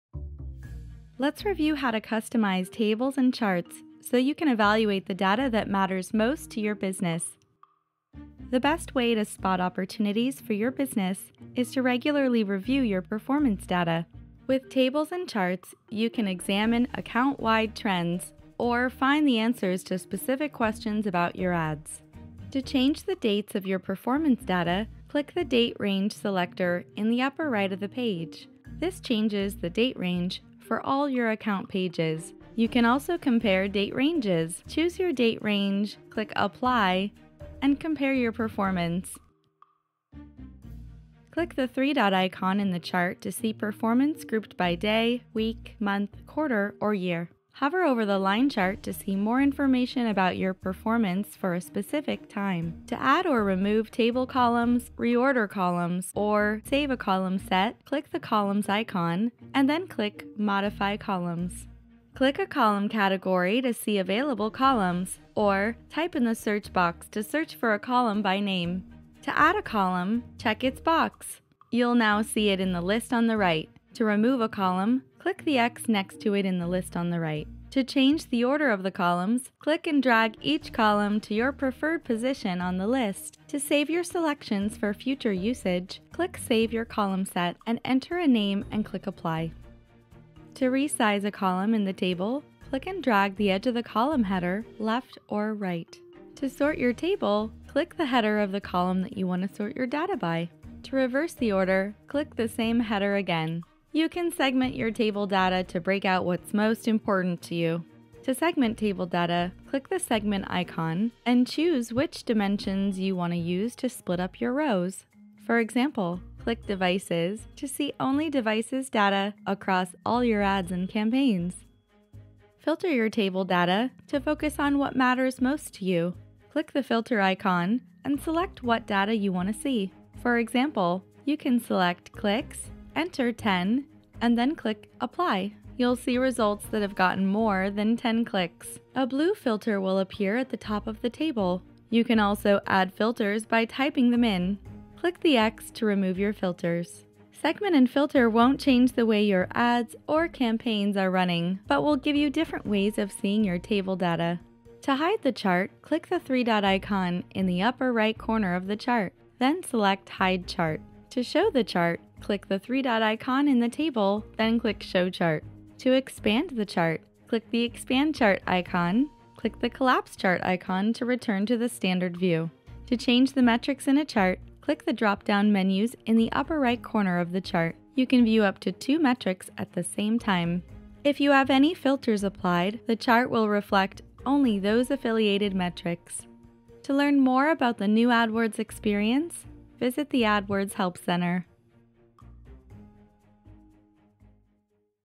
How to customize tables and charts in the new AdWords experience
Subtitles are available for this video. Learn how to turn on video captions

Learn more about customizing your tables and charts, so you can view the data that matters most to your business.